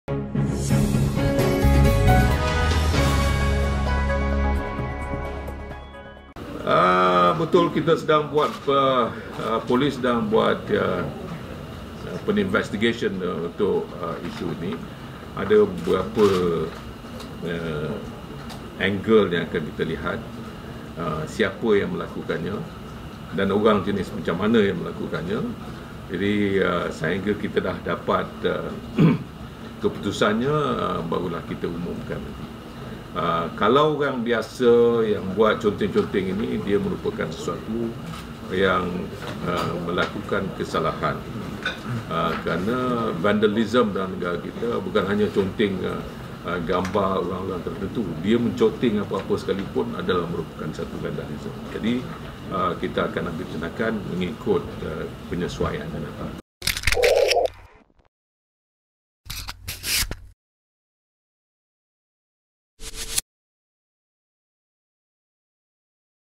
TERKINI : Polis Akan Siasat Pelaku Conteng Mural - Hamzah Zainuddin
Polis akan menyiasat daripada pelbagai sudut berhubung tindakan individu yang menconteng mural barisan pemimpin negara di Shah Alam semalam.

Menteri Dalam Negeri, Datuk Seri Hamzah Zainuddin berkata, langkah itu adalah bagi mengenalpasti tindakan yang sesuai dikenakan ke atas pelaku selain meneliti latar belakang serta motif ia dibuat.